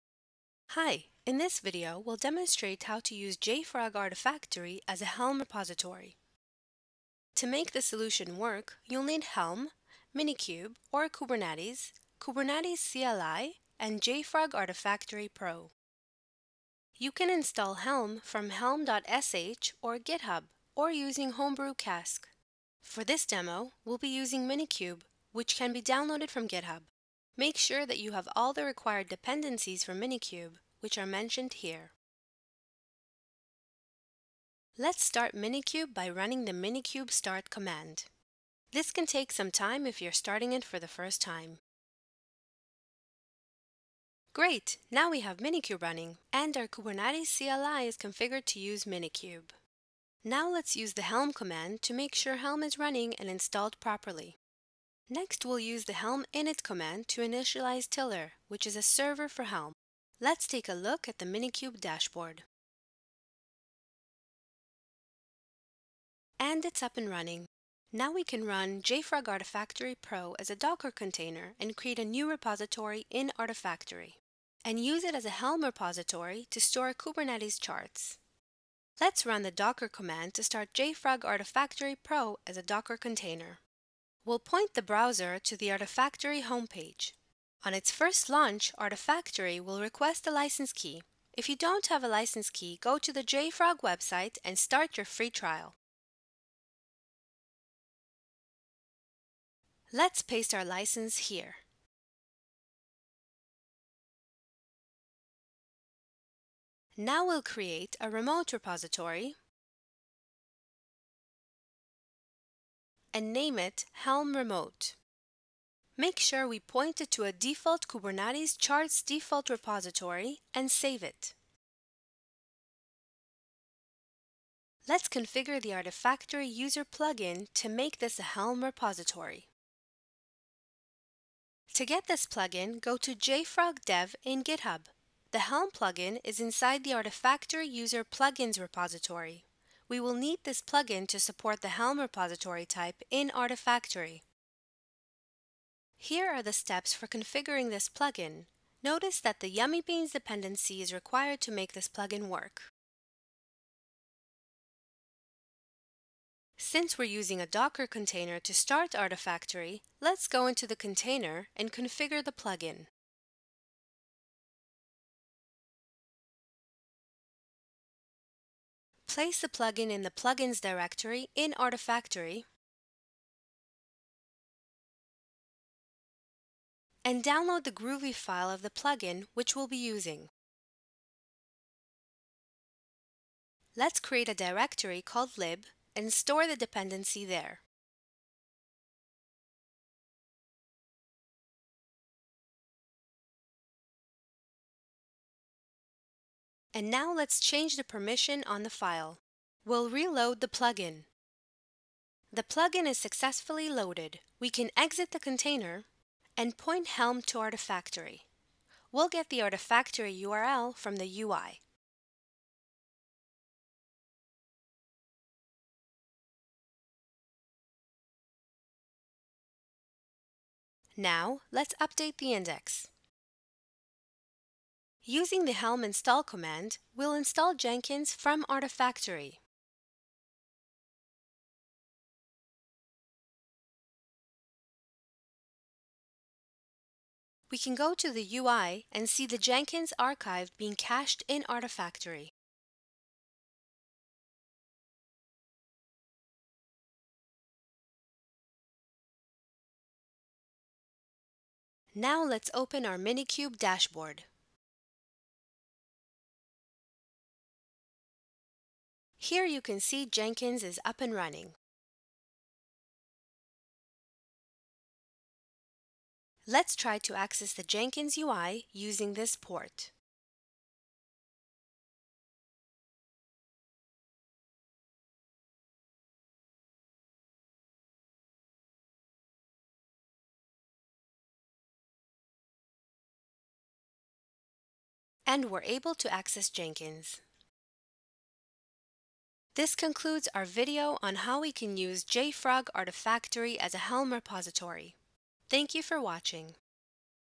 [Screencast] Helm Support in JFrog Artifactory
Watch this screencast to get a developers perspective on how to support Helm repositories within JFrog Artifactory.